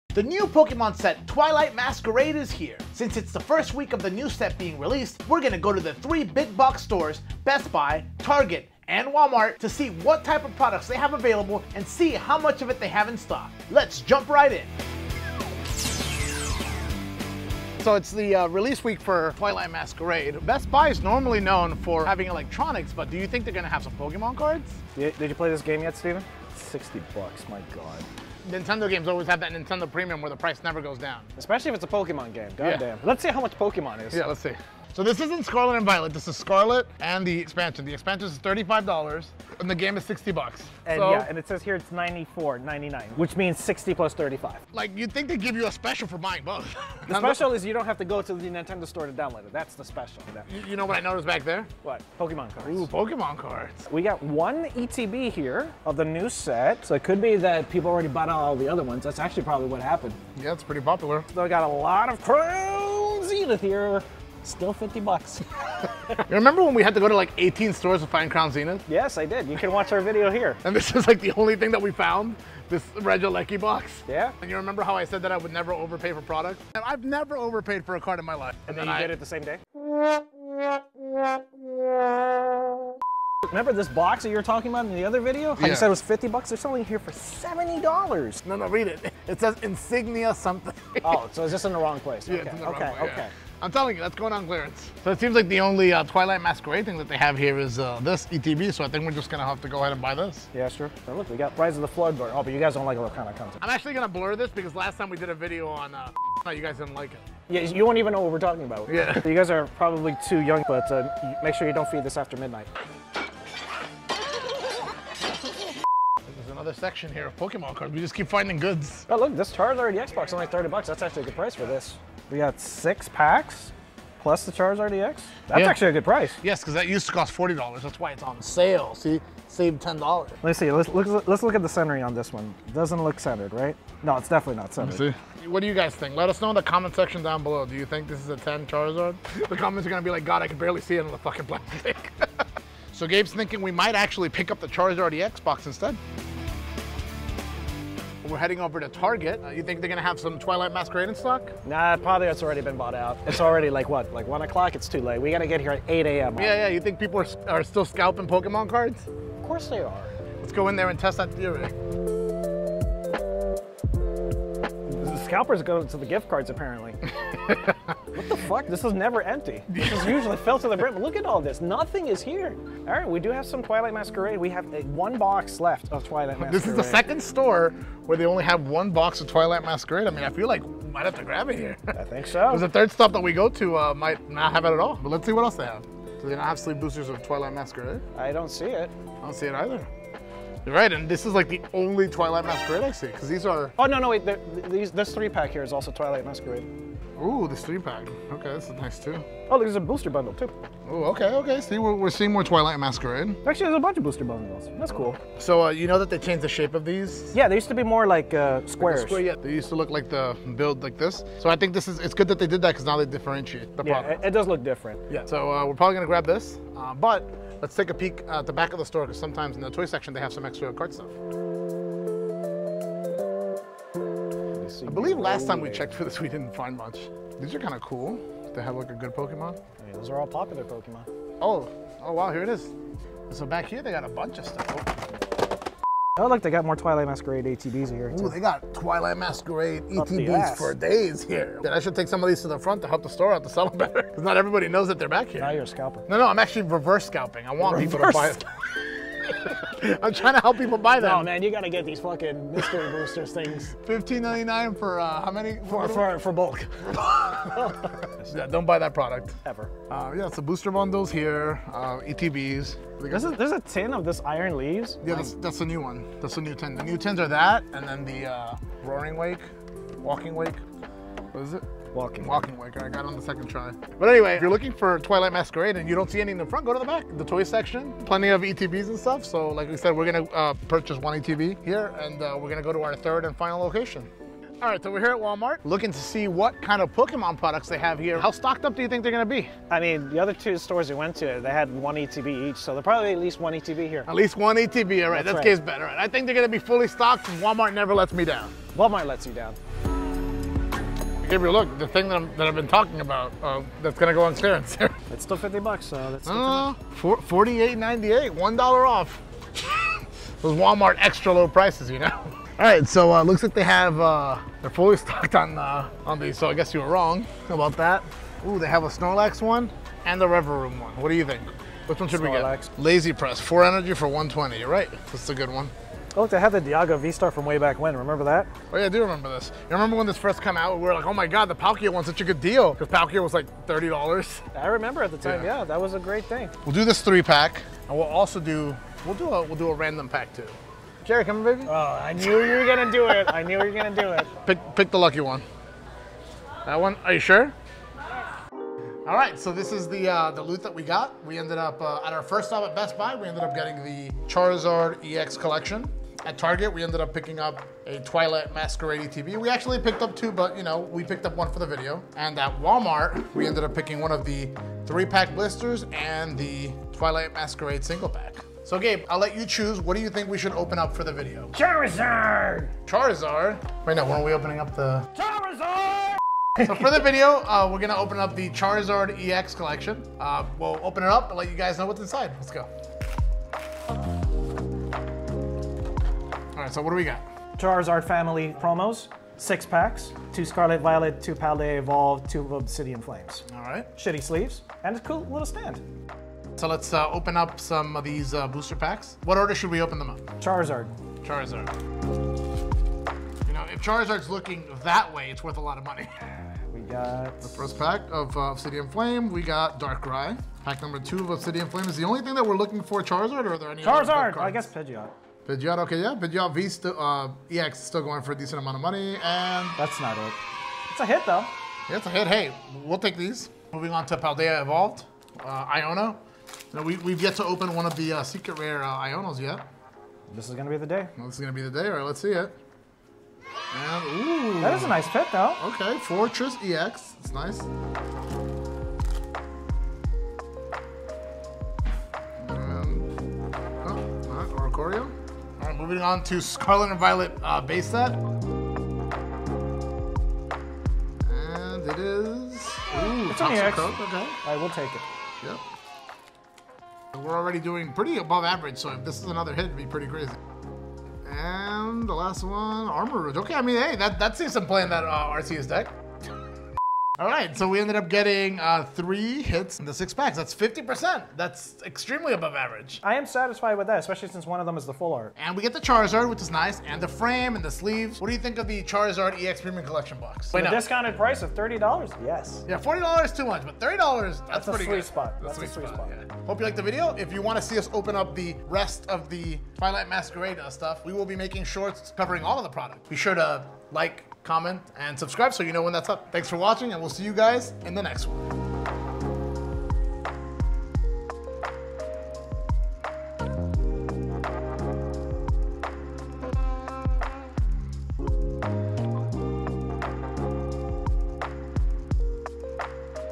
Where to find all the different Pokémon Twilight Masquerade Products?
In this video we go to three different stores, Best Buy, Target, and Walmart. To find the new Pokemon product, Twilight Masquerade!